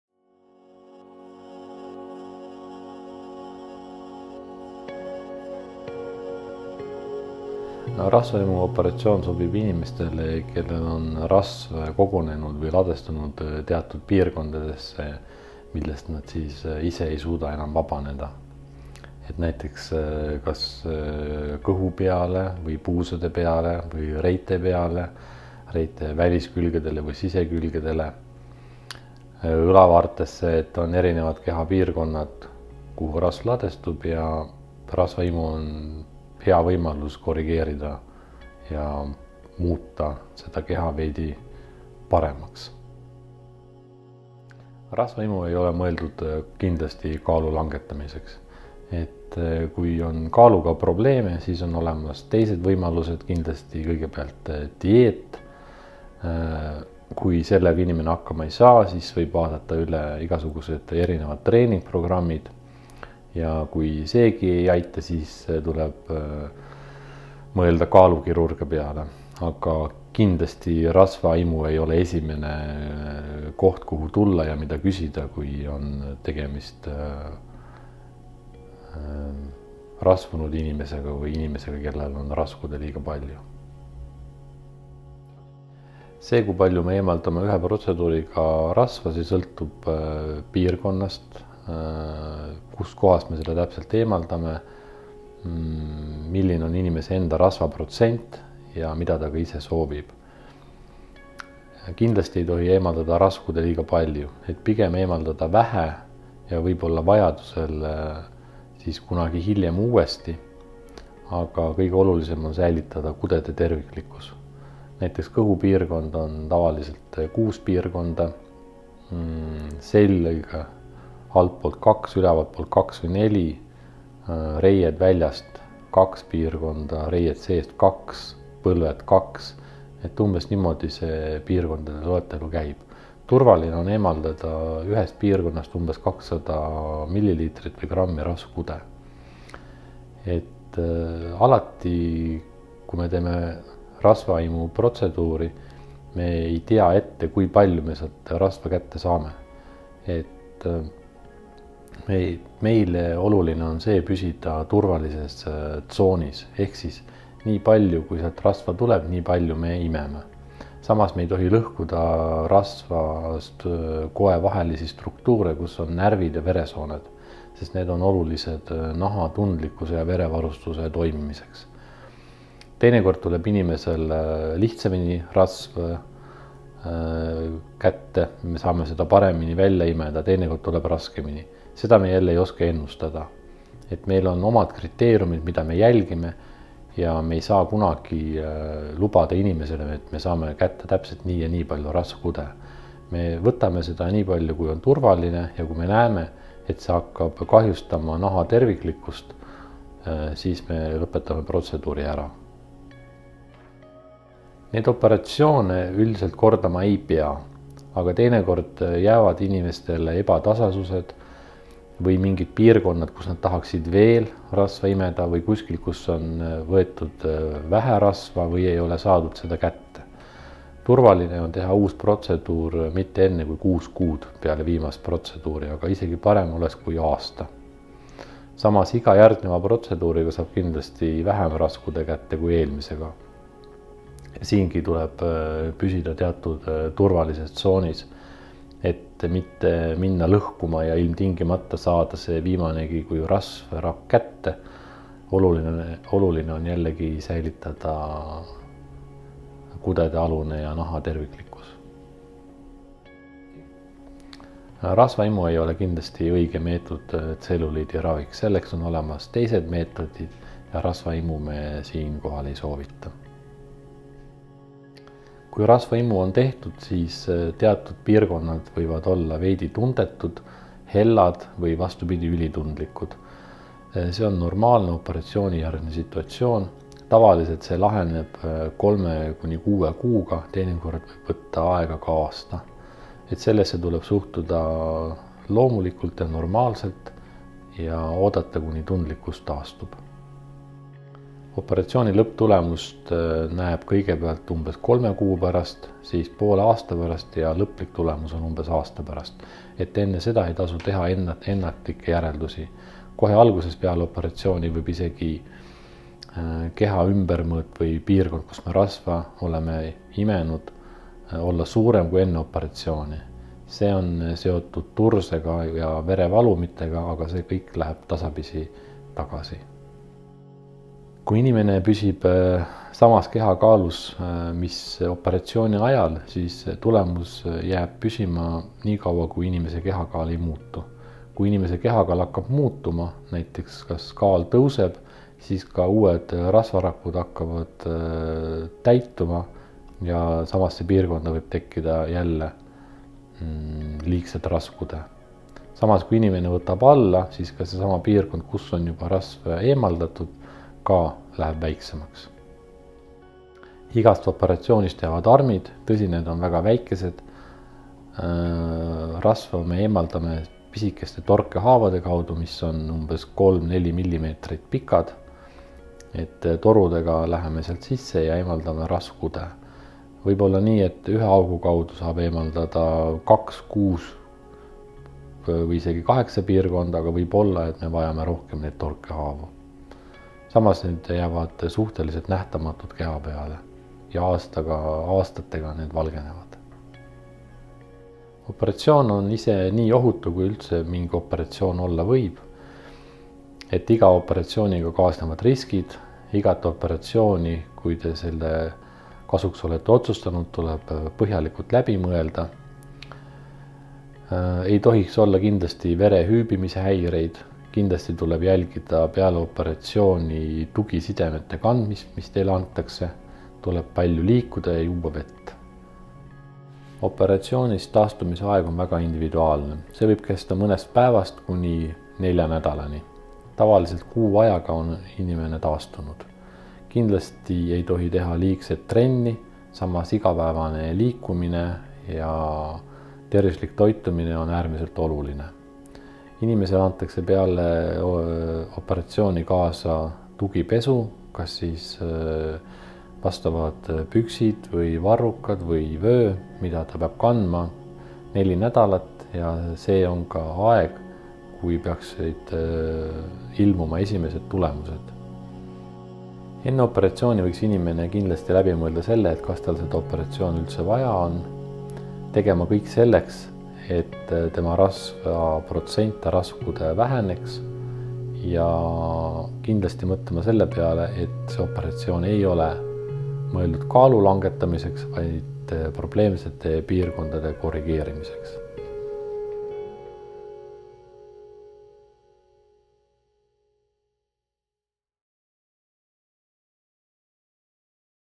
Rasvaimu - levinud küsimused. Dr. Siim Simmo - Health Clinic
Health Clinicu kirurg dr. Siim Simmo vastab kõige sagedasematele küsimustele, mis seoses rasvaimu operatsiooniga tekkida võivad.